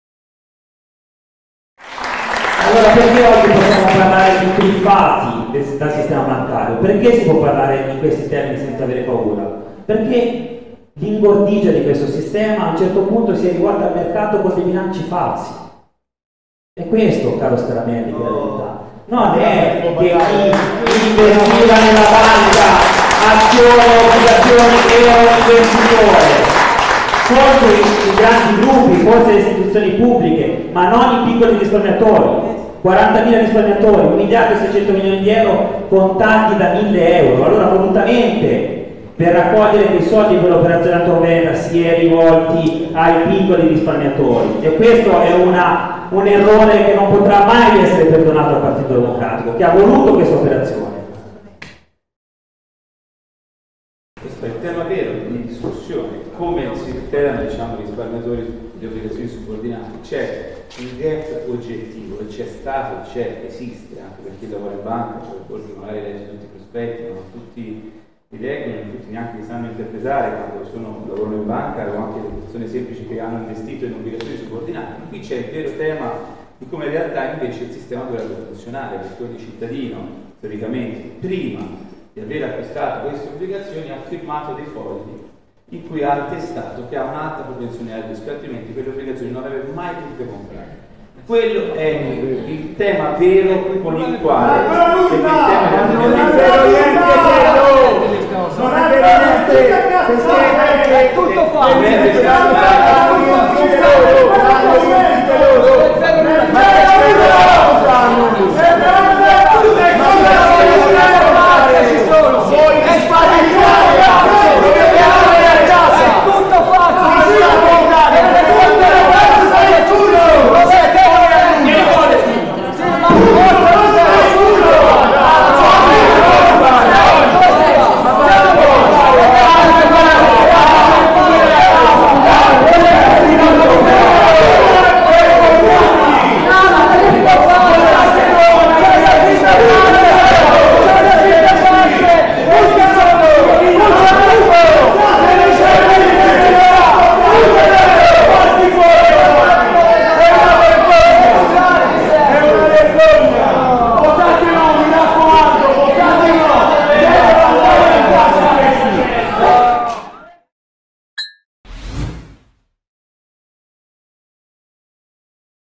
MPS: ESPLODE LA RABBIA DEI PICCOLI RISPARMIATORI
A tutto c'è un limite, ma loro lo oltrepassano sempre ... e i risparmiatori esplodono.
Guardate qua!